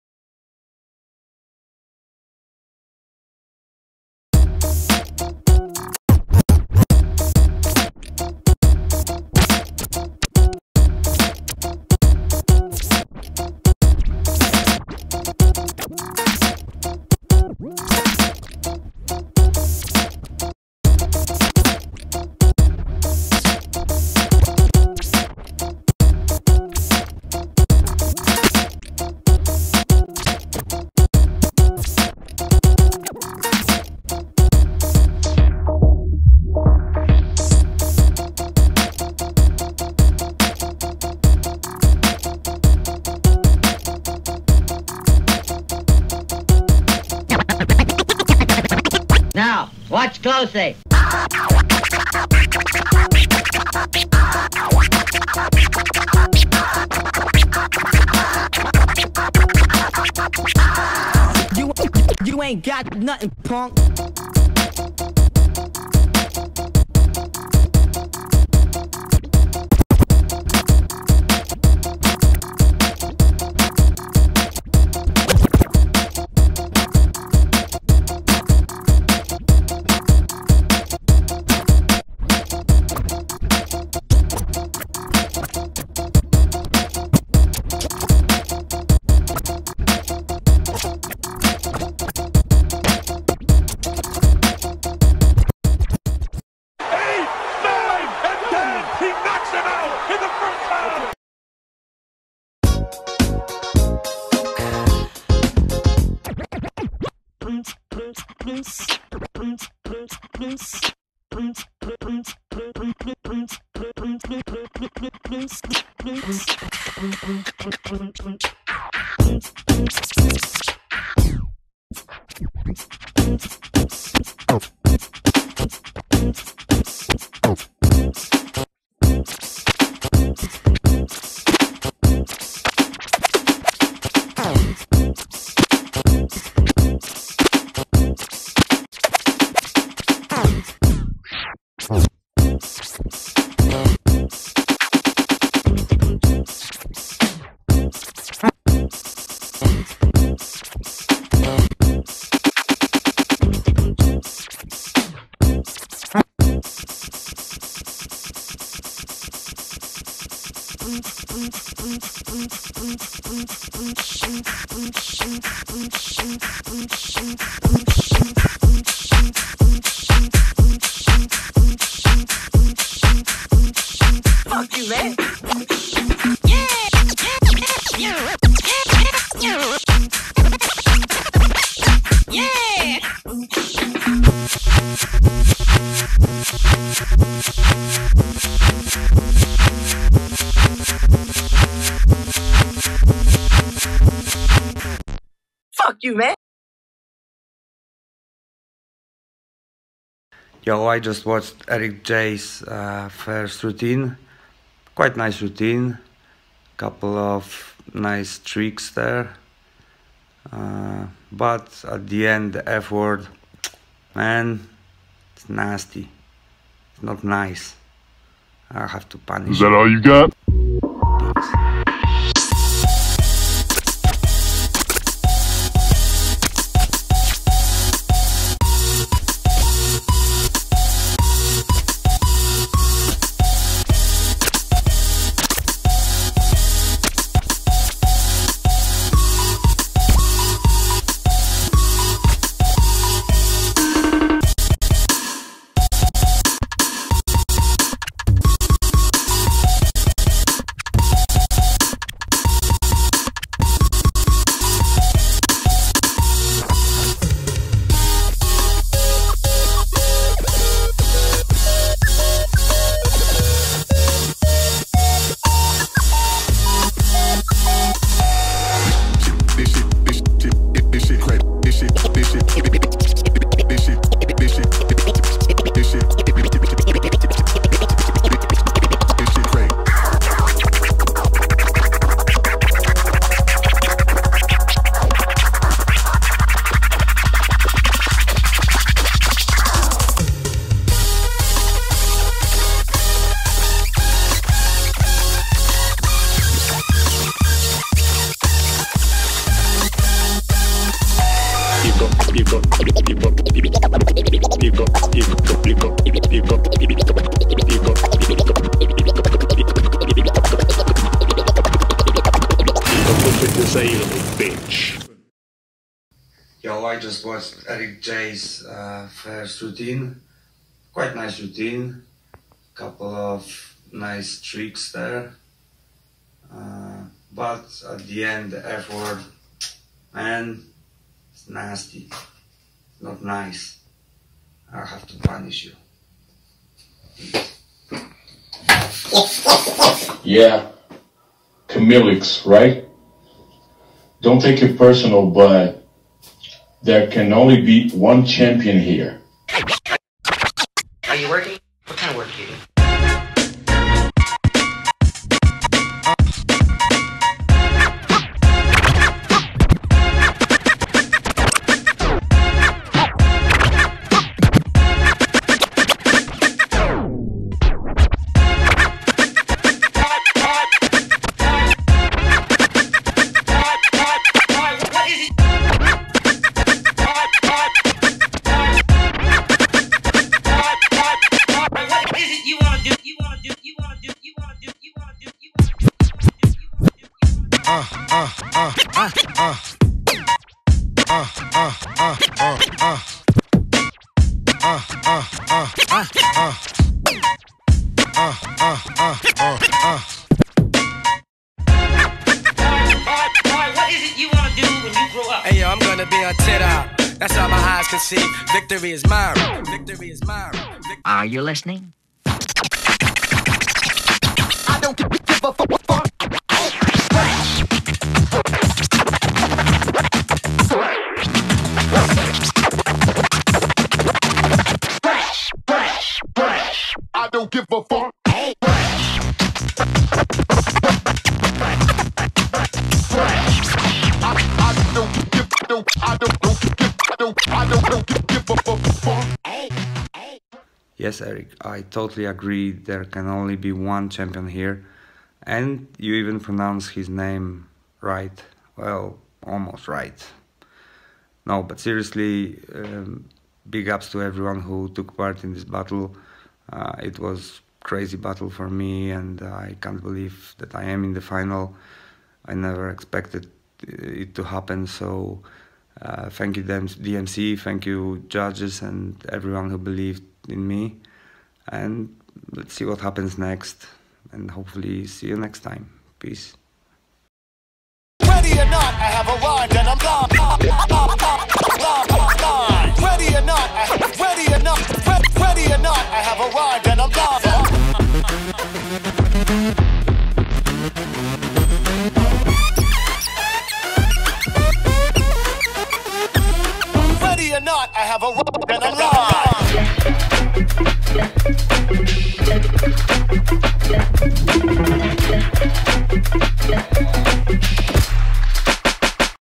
CHMIELIX (Poland) vs ERICK JAY (Brazil): 2021 DMC Battle for World Supremacy Finals
CHMIELIX (Poland) vs ERICK JAY (Brazil)
2021 DMC Battle for World Supremacy Finals

Erick Jay wins his 3rd DMC World title!

Hosted by DJ Nelson (2011 DMC Battle For World Supremacy Champion)
for more info!